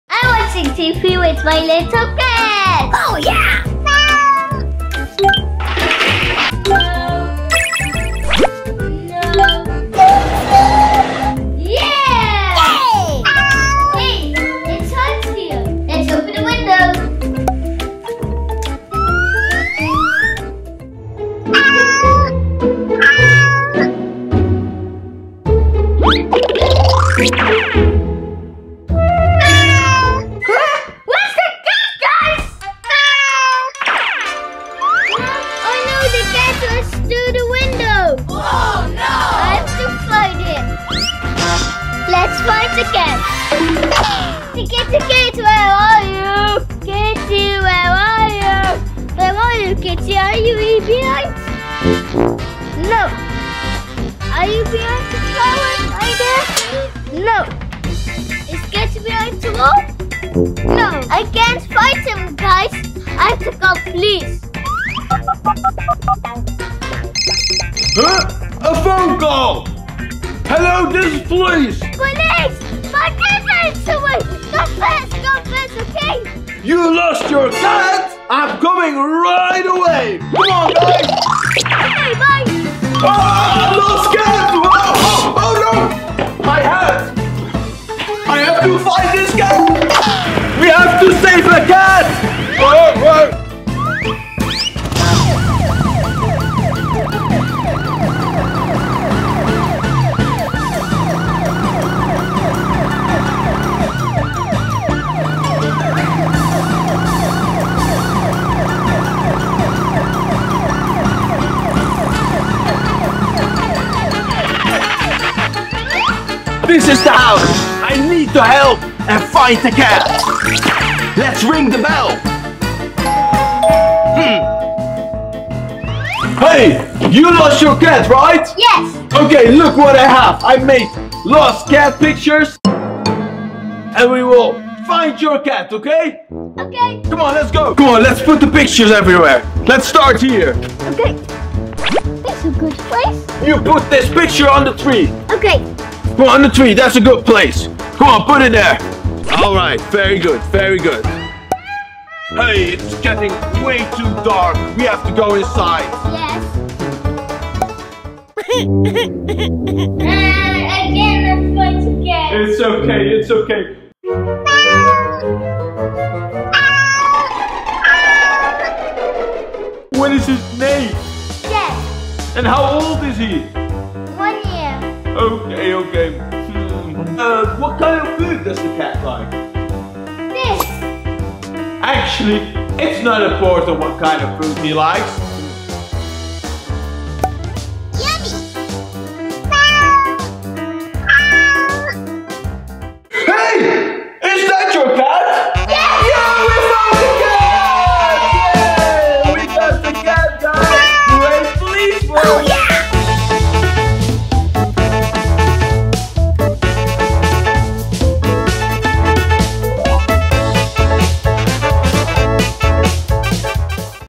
Jason finds lost cat with police
When Jason opens the window in house, our little cat escapes and runs outside! Jason tried to find the cat, but could not find the cat. So he calls his friend from the police to help searching and finding the cat!